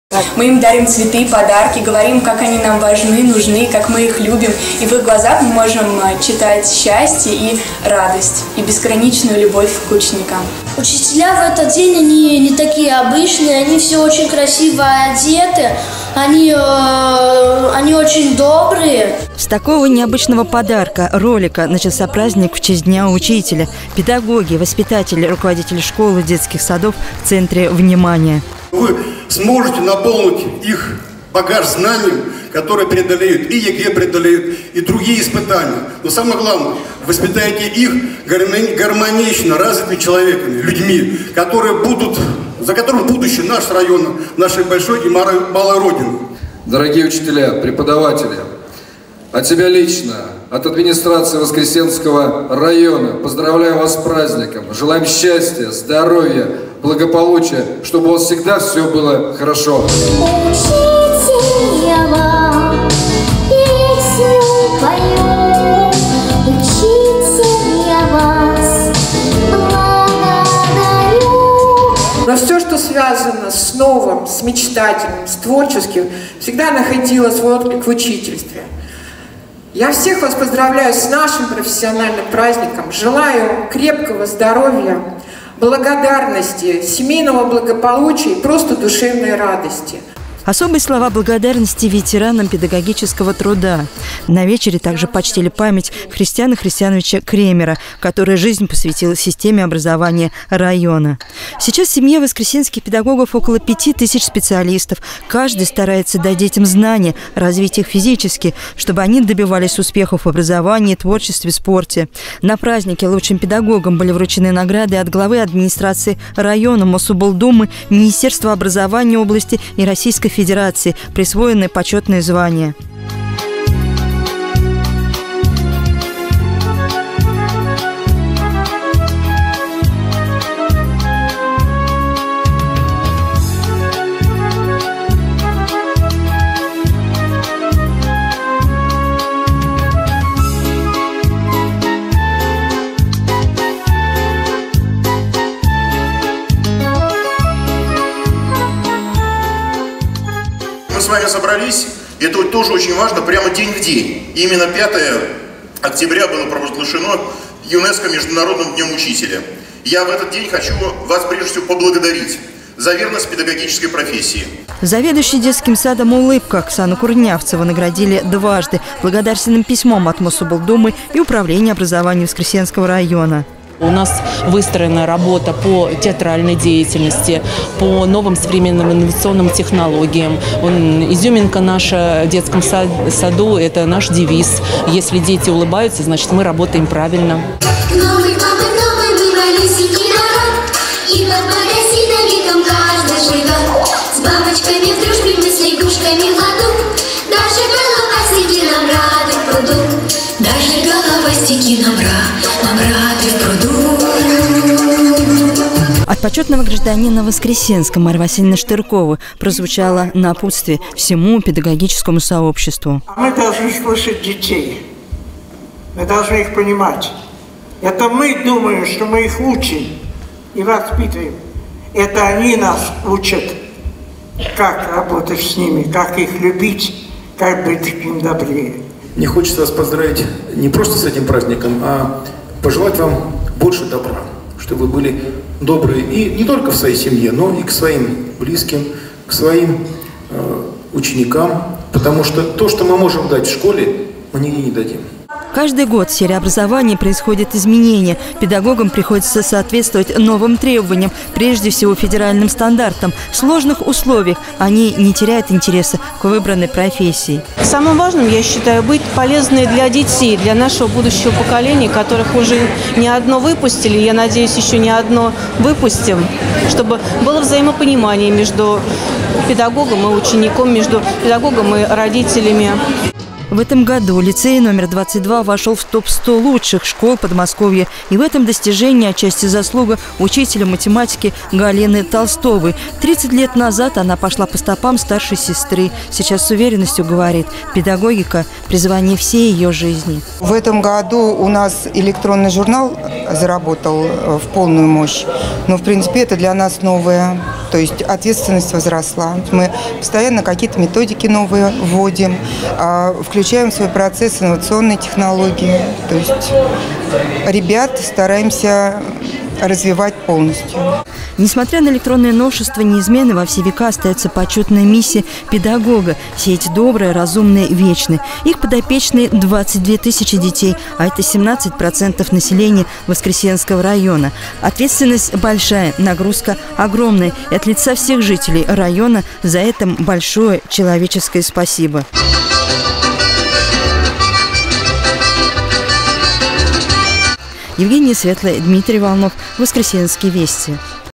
Во дворце культуры Юбилейный состоялось торжество, посвященное Дню учителя
Торжество в честь Дня учителя стало подарком за нелегкий труд. На празднике около 200 из них были отмечены наградами федерального, областного и районного уровней. Подробнее о вечере в сюжете нашего корреспондента.